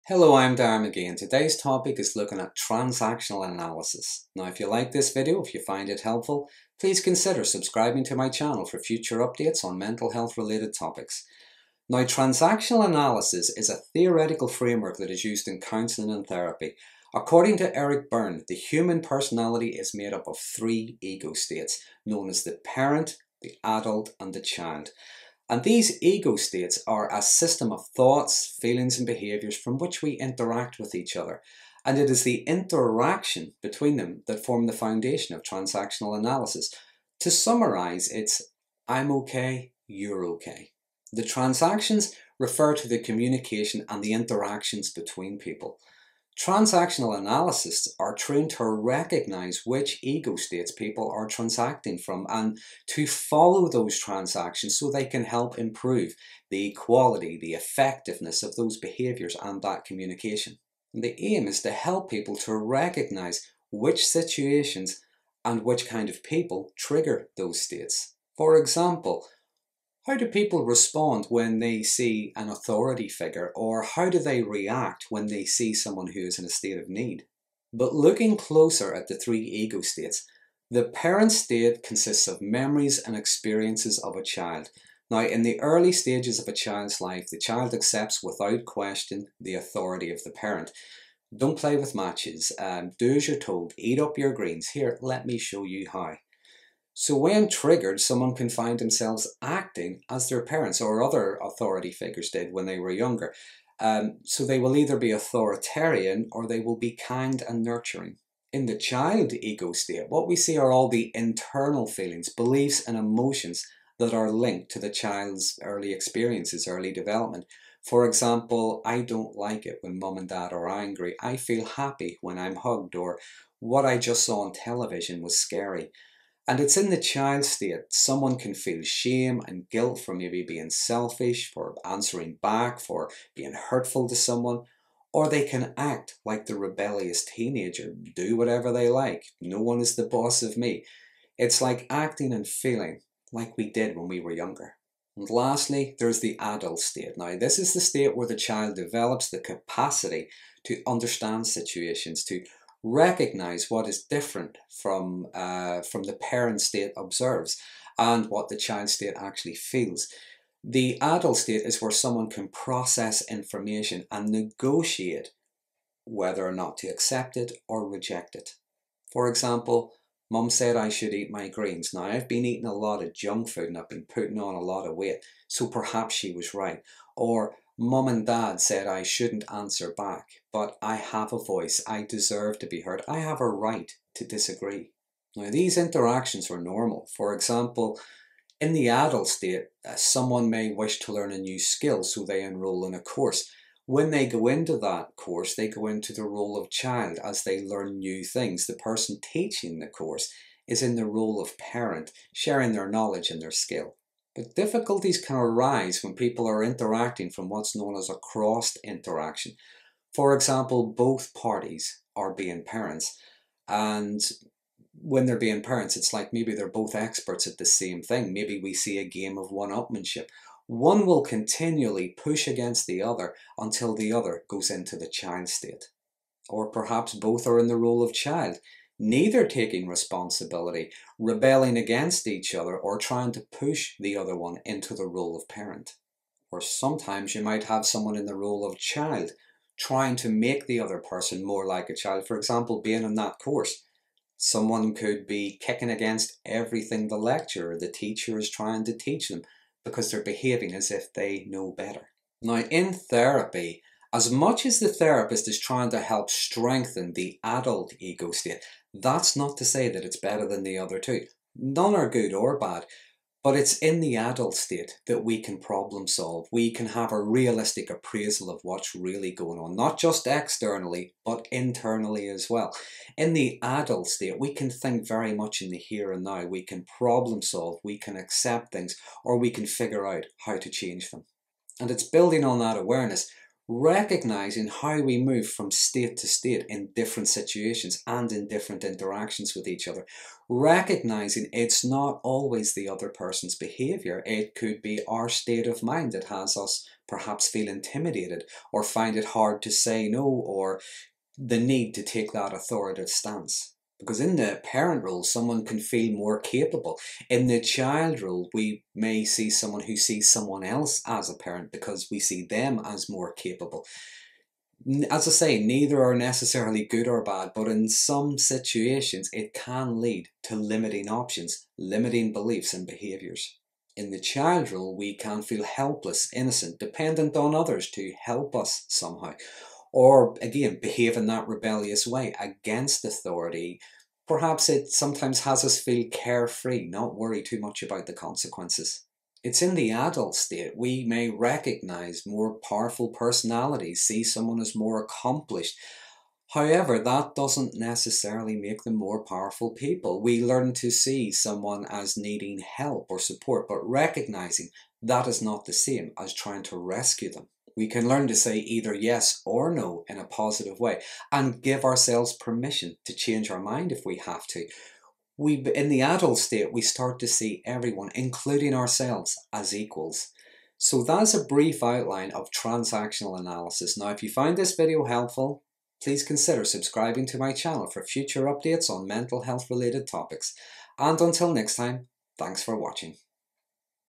Transactional Analysis
Transactional analysis is a framework used in counselling and therapy to recognise the interactions between people. According to Eric Berne we have three ego states, parent, adult and child, and transactional analysis looks at hw we go into different ego states in our interactions with certain people and in certain situations.
In this video Darren Magee gives a brief outline of the theory and gives examples of the ego states.